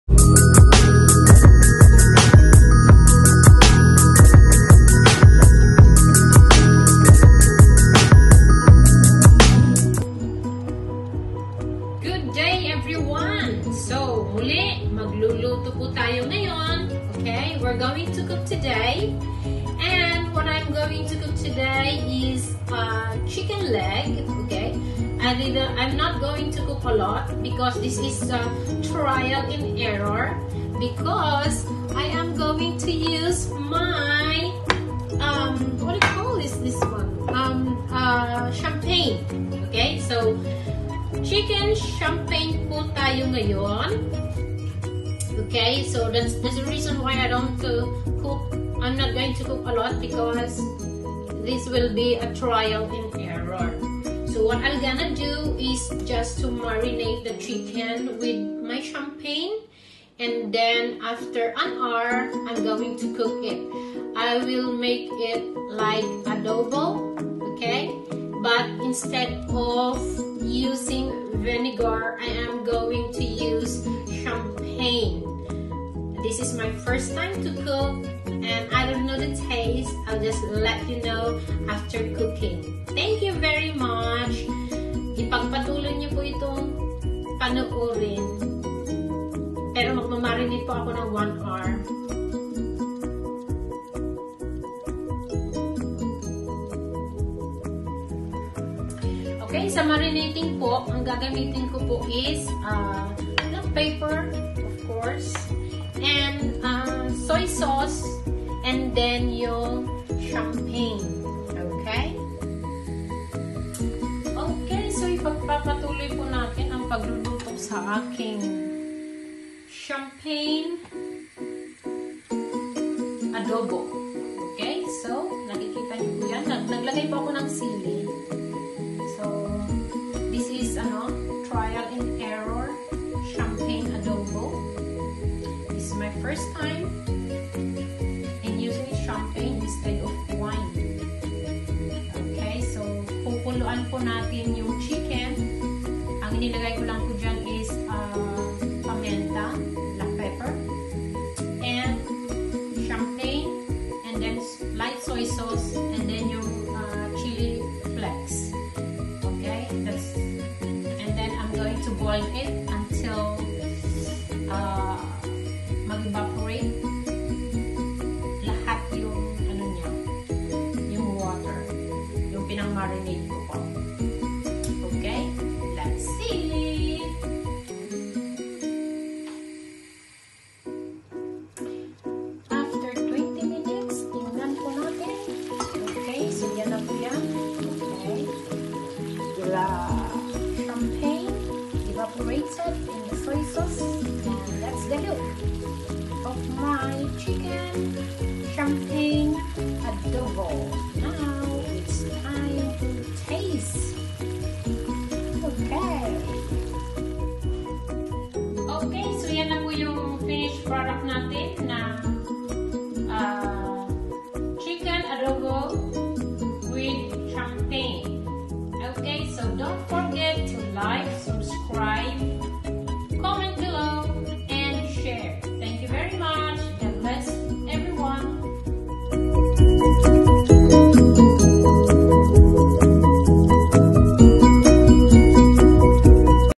How to cook champagne chicken Adobo.by: Shirley Caytap
For our champagne chicken adobo.
You needed
Chicken of your choice
Marínate the chicken with
1 cup of champagne
1 teaspoon of ground block pepper
1/4 cup of soy sauce
Then boil the marinated chicken until the sauce thickens and meat is tender.